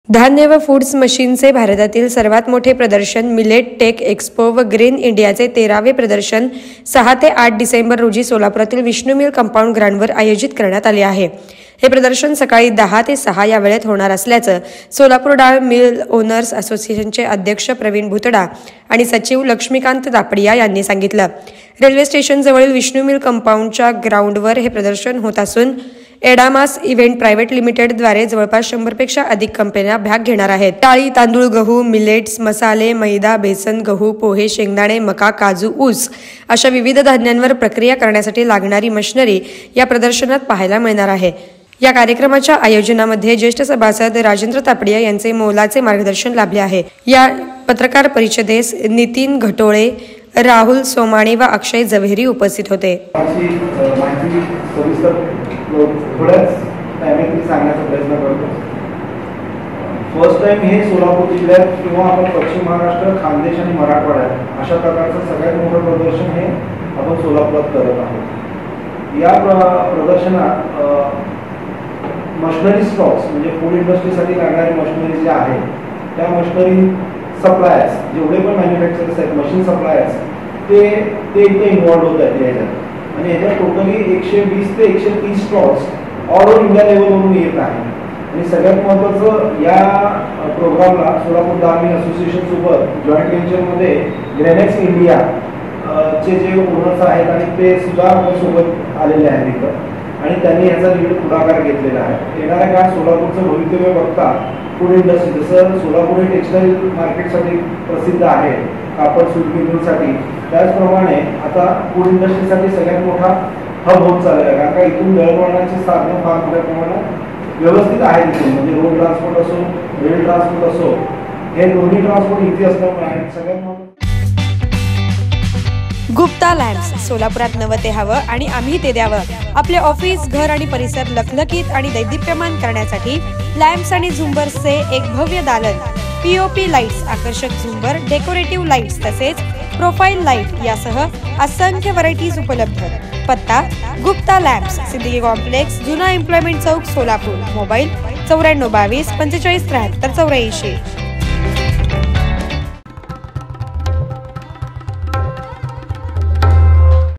सोलापुरात ६ ते ८ डिसेंबर दरम्यान धान्य व फुड्स मशिनरीचे प्रदर्शन
धान्य व फुड्स मशीनचे भारतातील सर्वात मोठे प्रदर्शन मिलेट टेक एक्सपो व ग्रेन इंडियाचे १३ वे प्रदर्शन ६ ते ८ डिसेंबर रोजी सोलापुरातील विष्णू मिल कंपाउंड ग्राउंडवर आयोजित करण्यात आले आहे. हे प्रदर्शन सकाळी १० ते ६ या वेळेत होणार असल्याचे सोलापूर डाळ मिल ओनर्स असोसिएशनचे अध्यक्ष प्रवीण भुतडा आणि सचिव लक्ष्मीकांत तापडिया यांनी सांगितले.